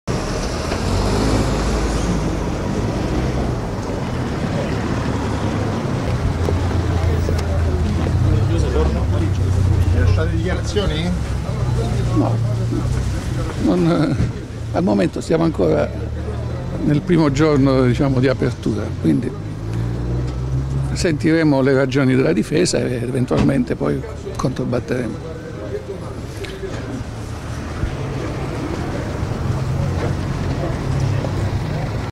Ginnastica, iniziato il processo sportivo Maccarani per maltrattamenti
Il procuratore Rossetti: "Sentiremo ragioni della difesa e controbatteremo"

 (Roma). E' iniziato il processo sportivo a Roma a carico dell'ex neo direttore tecnico della squadra nazionale di ginnastica ritmica, Emanuela Maccarani, e la sua assistente, Olga Tishina. Le prime ad arrivare dinnanzi al Tribunale federale della Federginnastica sono state le due imputate che non hanno voluto rilasciare dichiarazioni in ingresso. Subito dopo il procuratore Michele Rossetti che ha sottolineato "al momento siamo al primo giorno, quello di apertura. Sentiremo le ragioni della difesa ed eventualmente controbatteremo". Le due allenatrici dall'autunno scorso sono al centro della vicenda legata a presunte vessazioni da parte di alcune loro ex allieve. (Davide Di Carlo/alanews)

al momento siamo ancora nel primo giorno diciamo di apertura quindi sentiremo Le ragioni della Difesa eventualmente Puoi controbattere